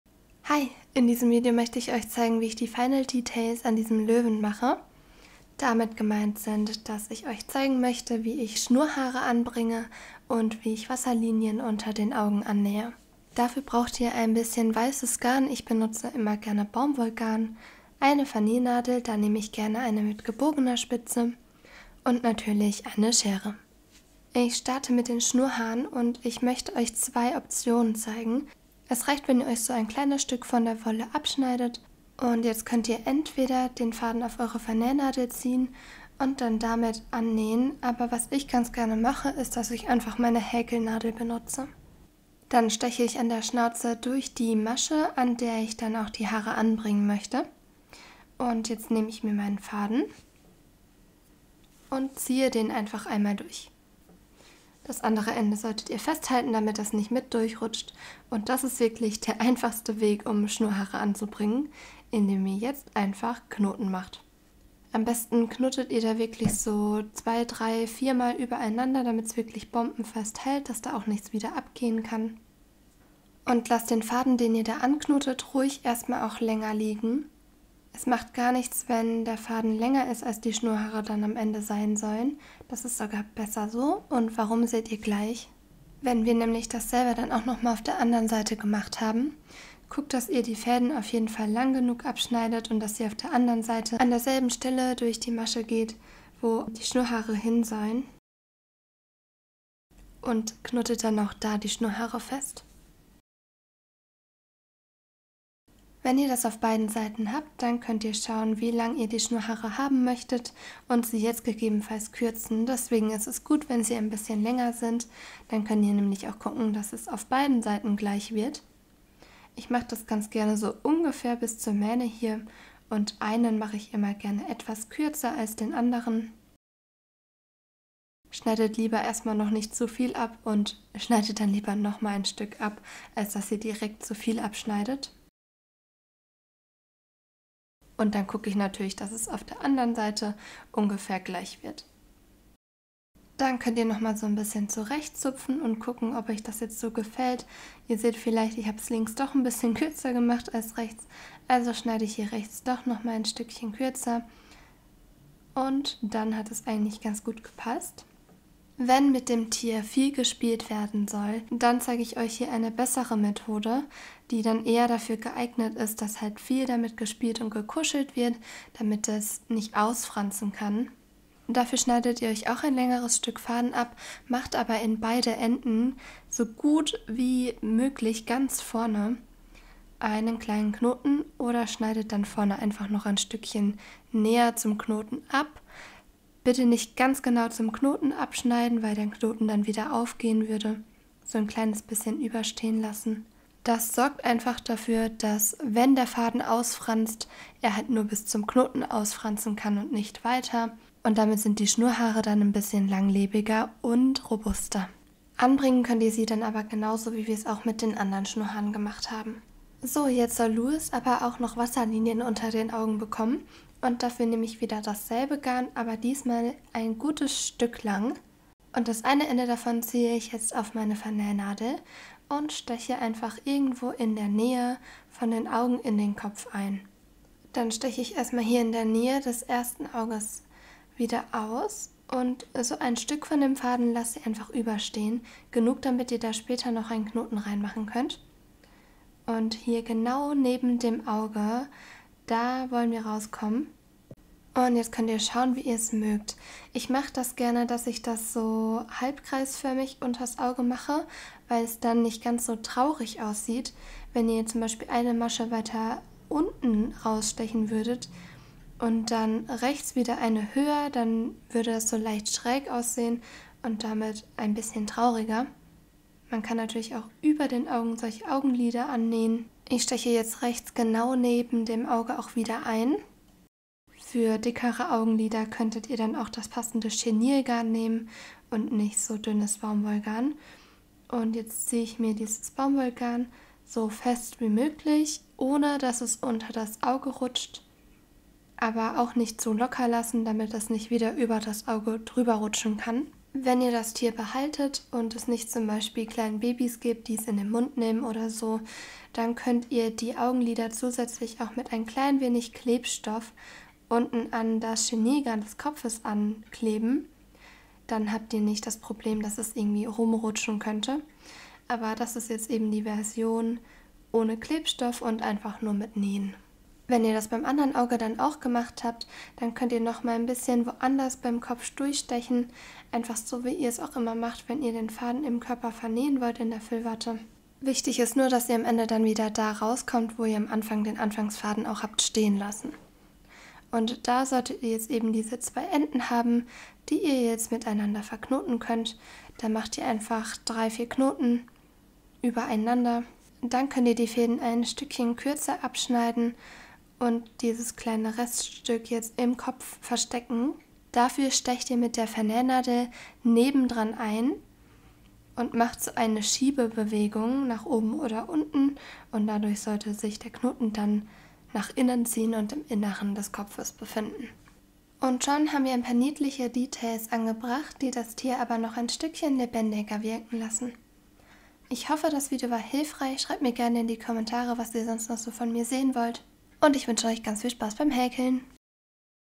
Schnurrhaare und Wasserlinien bei Amigurumis annähen | Final Details nähen
In diesem Video zeige ich dir zwei Methoden, wie man wie man Schnurrhaare bei Amigurumis annähen kann. Eine Methode für Tiere mit denen nicht gespielt wird und eine sichere und langlebige Methode für Tiere mit denen viel gespielt und gekuschelt werden soll.
Außerdem zeige ich dir, wie man Wasserlinien annäht, um die Tiere lebendiger wirken zu lassen.

Liebe Grüße, Laura.

Instagram ☞   / lanufaktur
TikTok ☞   / lanufaktur

(Werbung)